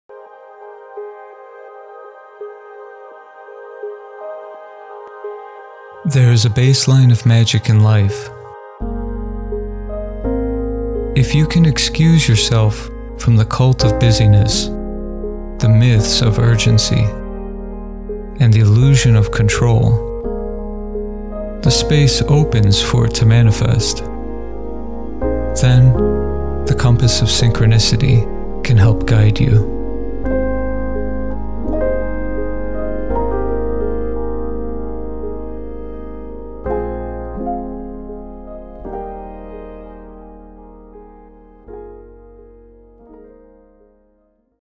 Compass of Synchronicity
poem and voice by Todd Fink
animation and illustration by Natalia Maca